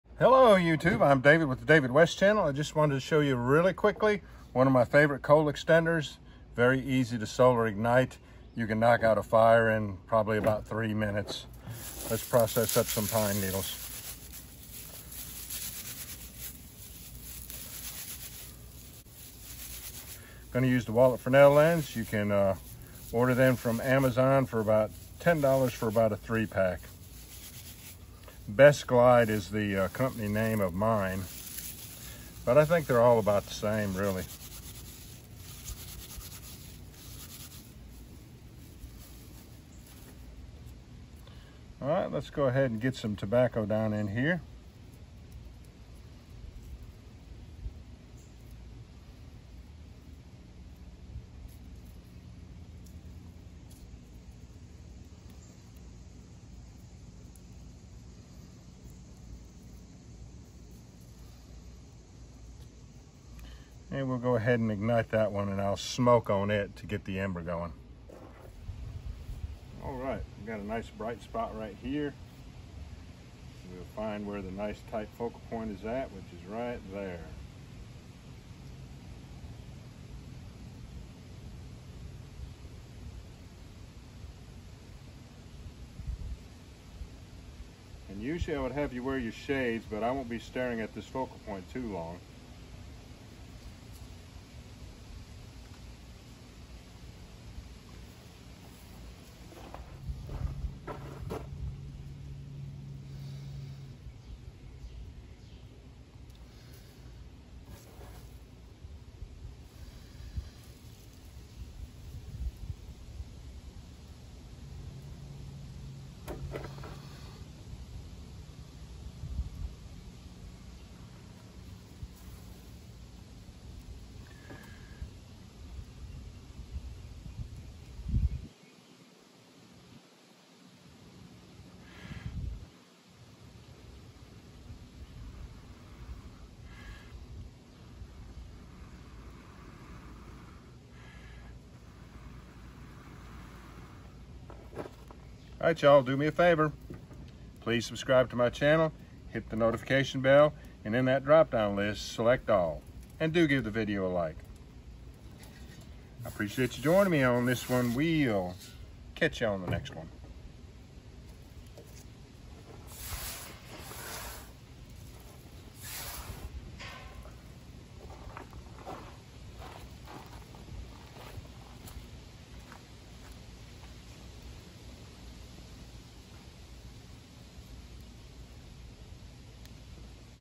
Solar Ignited Cigarette Butt Fire
A 3 pack of Best Glide Wallet Fresnel Lenses cost $10 on Amazon. Today, a quick solar ignition of a cigarette butt which ignites Pine Needles. Quick and easy... Let's get back into the air conditioning.